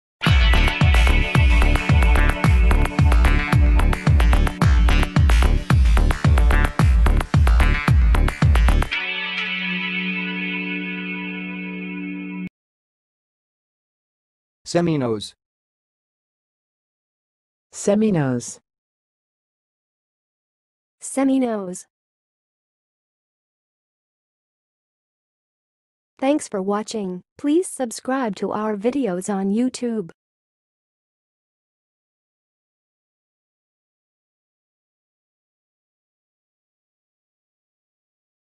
SEMINOSE - HOW TO PRONOUNCE IT?
SEMINOSE - full guide about Seminose in 2023

How do you pronounce 'Seminoles'?
What are the alternative pronunciations of 'Seminoles'?
Can you provide a phonetic breakdown of the word 'Seminoles'?
SEMINOSE Meaning And Definition:
Seminoles are a Native American tribe originally from Florida.
They are part of the Creek Confederacy and have their own language, culture, and traditions.
SEMINOSE Pronunciation:
The pronunciation of 'Seminoles' is suh-muh-nohls, but it can also be pronounced sem-uh-nohls or see-muh-nohls.
Use SEMINOSE In Your Speech:
1. The Seminoles have a rich oral tradition passed down through generations
2. The Seminole Tribe of Florida operates several successful casinos
3. Many Seminoles still practice traditional crafts like pottery making
Please answer in comments for the most popular SEMINOSE questions:
1. What role did the Seminoles play in the Creek Confederacy?
2. How did the Seminoles adapt to the Florida environment?
3. What is the significance of the Seminole Tribe's casinos?